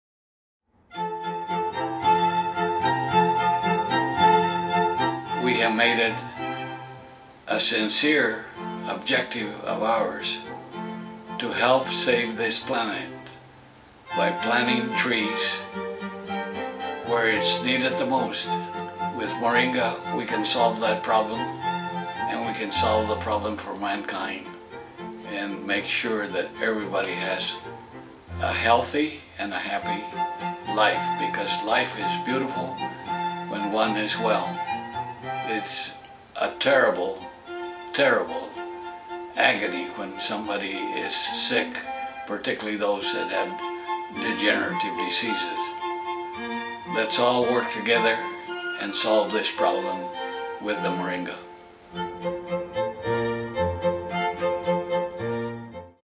Moringa with Dr. Sanchez
Dr. Albert Sanchez Sr. Talks about the Moringa Tree